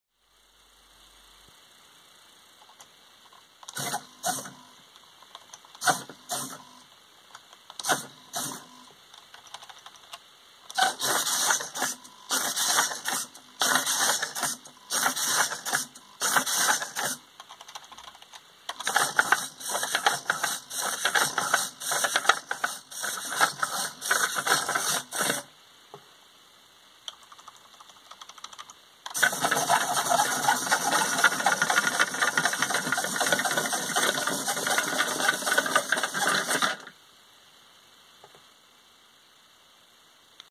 「youtuashi」obakabotya
GUGEN2018の応募作成品のテスト映像です。
「よつあし」にハロウィンバケツをかぶせただけなのですが、可愛く動きます。

です。